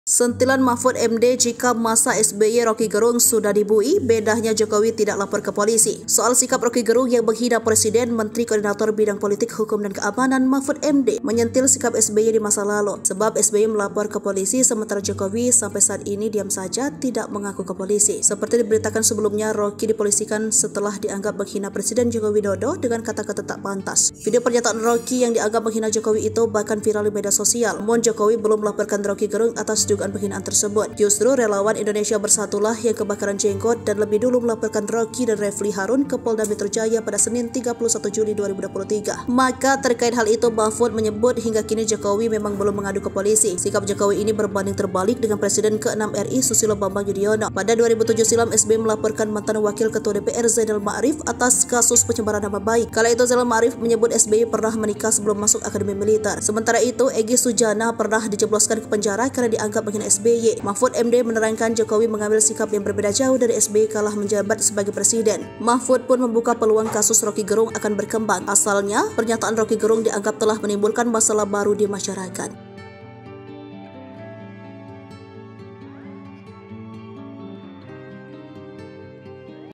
Sentilan Mahfud MD, Jika Masa SBY Rocky Gerung Sudah Dibui, Bedanya Jokowi Tidak Lapor ke Polisi
SOAL Sikap Rocky Gerung yang menghina Presiden, Menteri Koordinator bidang Politik, Hukum dan Keamanan (Menkopolhukam) Mahfud MD mnyentil sikap SBY di masa lalu.

Sebab SBY melapor ke polisi, sementara Jokowi sampai saat ini diam saja. Tidak mengaku ke polisi.

Seperti diberitakan sebelumnya, Rocky Gerung dipolisikan setelah dianggap menghina Presiden Joko Widodo (Jokowi) dengan kata-kata tak pantas.

Lihat informasi terkini bersama kami, di:

Palembang.tribunnews.com